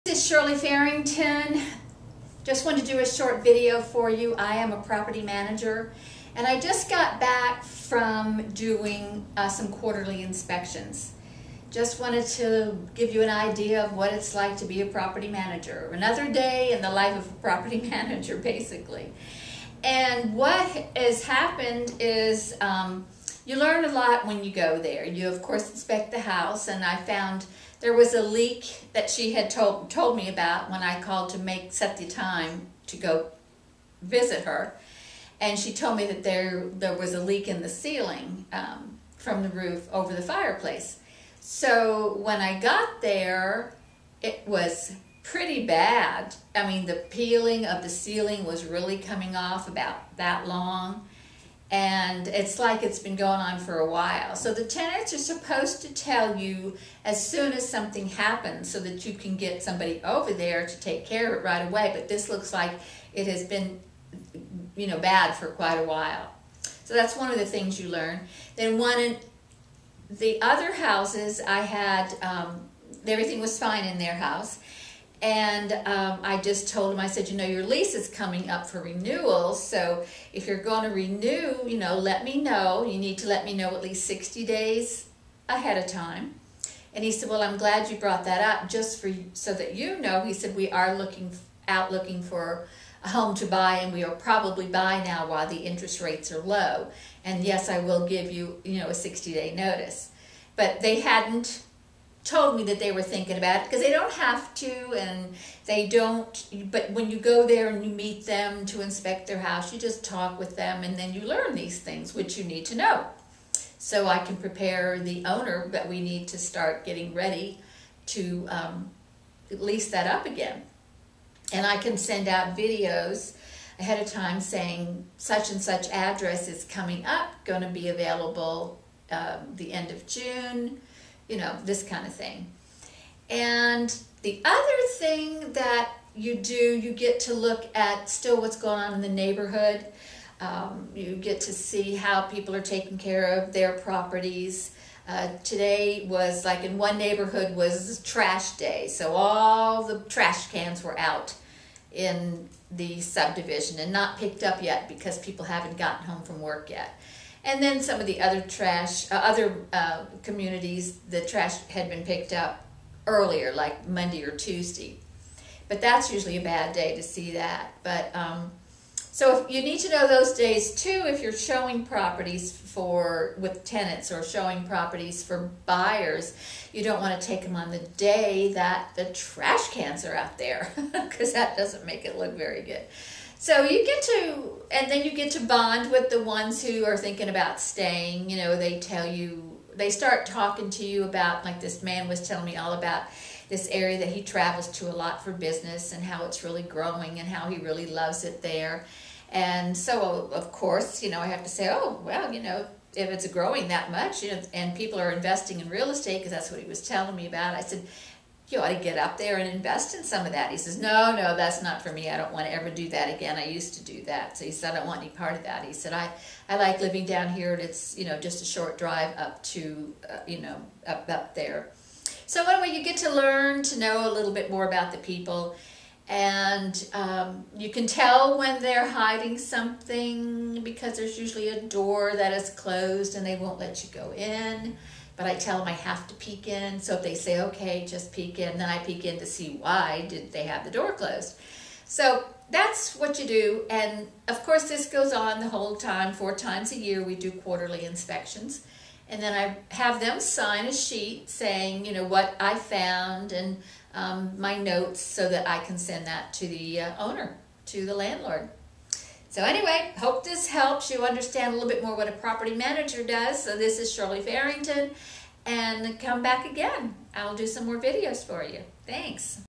Shirley Farrington, The Property Manager Consultant
Another Day in the Life of A Property Manager! Here's a little help in what I do as a property manager. This is also why owner/landlords need to do those quarterly inspections. I have spoken with more owner/managers say they never do inspections. In addition they are surprised how bad the property was left in when they left. So this is just one tip on why you should do your inspections on a regular basis. Shirley Farrington, Real Estate Agent Specializing in working with investors.